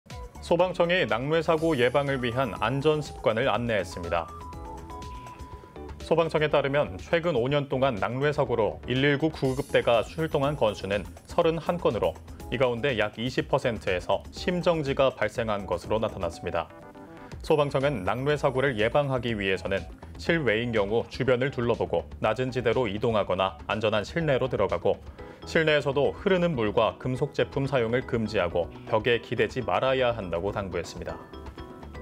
[소셜] 생활ㅣ소방청, 낙뢰 사고 예방 위한 안전 습관 안내 (이한승)ㅣCTS뉴스
소방청이 낙뢰 사고 예방을 위한 안전 습관을 안내했습니다.

소방청에 따르면 최근 5년 동안 낙뢰 사고로 119 구급대가 출동한 건수는 31건으로 이 가운데 약 20%에서 심정지가 발생한 것으로 나타났습니다.

소방청은 낙뢰 사고를 예방하기 위해서는 실외인 경우 주변을 둘러보고 낮은 지대로 이동하거나 안전한 실내로 들어가고 실내에서도 흐르는 물과 금속제품 사용을 금지하고 벽에 기대지 말아야 한다고 당부했습니다.